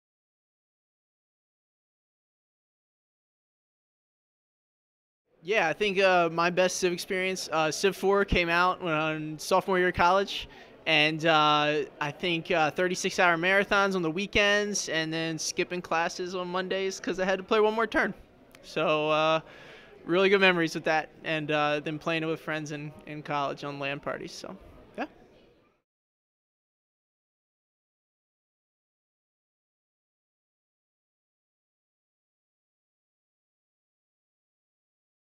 Civilization Moments - Nick
Wikia Super Fans dish on their favorite Civ Moments.